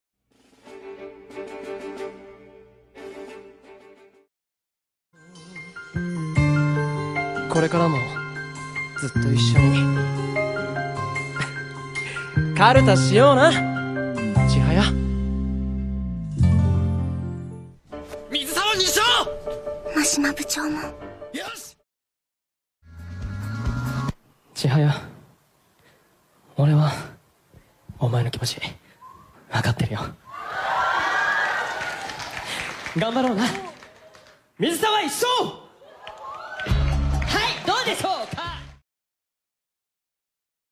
Yonaga Tsubasa impersonating Taichi Mashima from Chihayafuru
If you didnt know...
Yonaga Tsubasa also voices Komano Tsutomu/Tsukue-kun/Desktumo-kun.

Yonaga does really good impersonations of Miyano doesnt he...?
And it looks like he is a TaichixChihaya fan!!